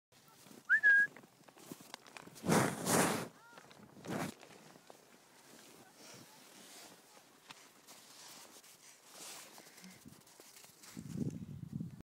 German Pinscher Bonette & snow
German Pinscher Bonette loves snow & wintertime